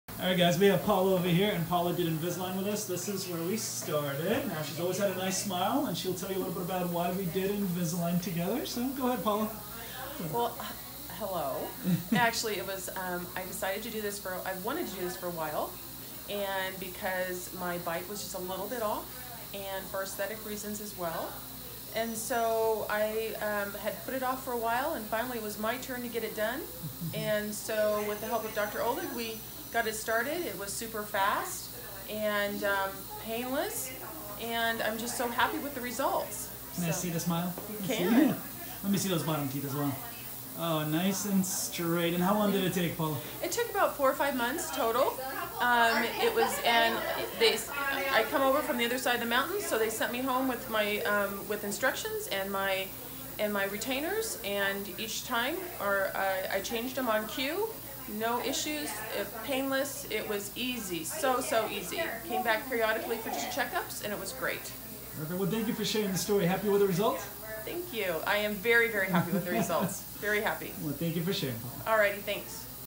Paula's Invisalign Testimonial
We get a lot of questions from patients of whether or not Invisalign treatment is actually feasible with us if they don't live locally. The answer is a resounding, YES!

Paula, who traveled from Eastern Washington to complete her Invisalign treatment with us, is a great testament for how easy it can be to seek treatment with us from afar. Listen to more of Paula's story to hear how she made it all work.